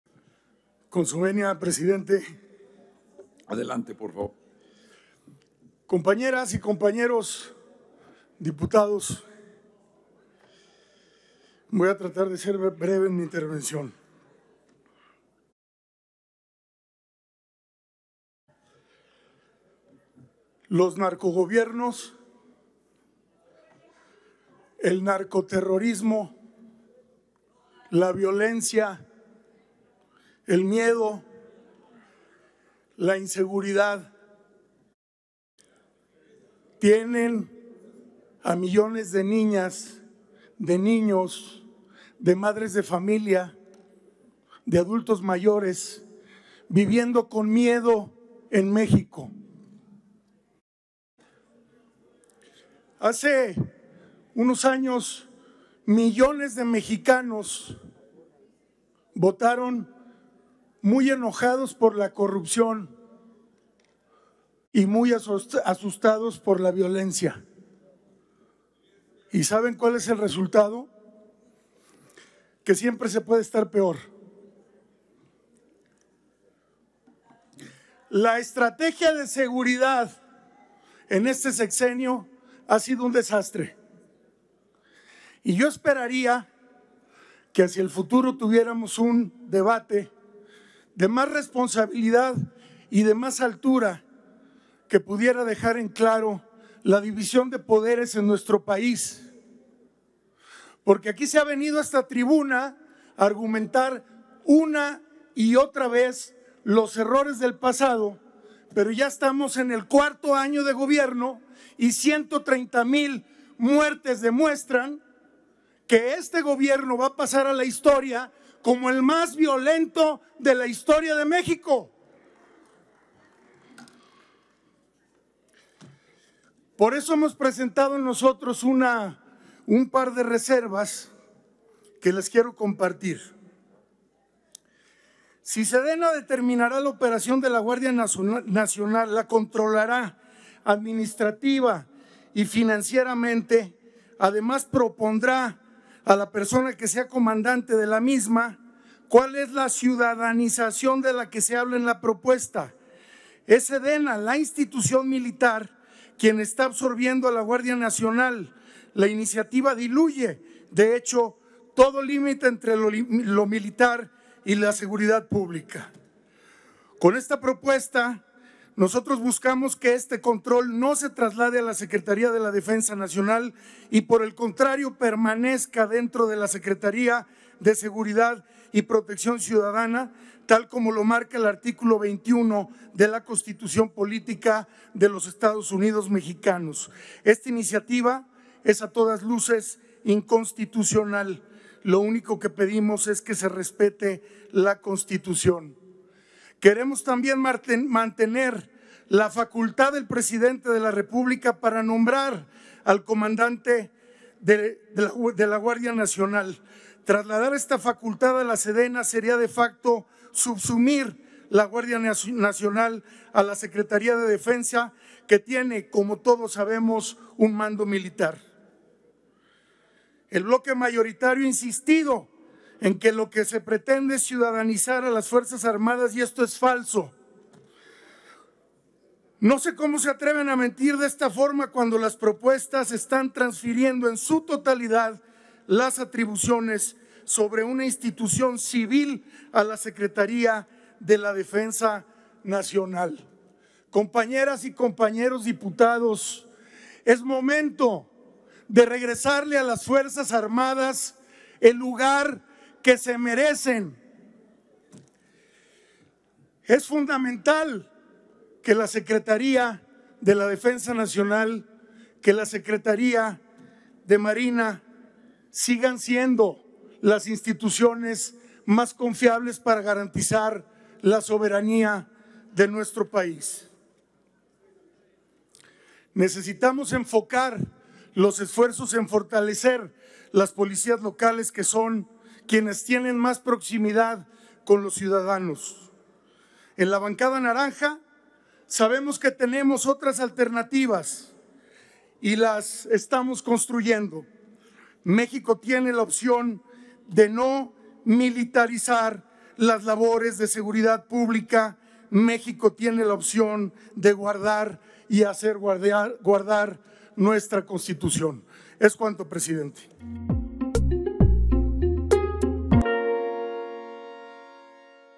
Dip. Manuel Jesús Herrera Vega (MC) / Presentación de reservas
El diputado Manuel Jesús Herrera Vega, de MC, presenta reservas al proyecto que modifica diversas leyes en materia de Guardia Nacional y Seguridad Pública.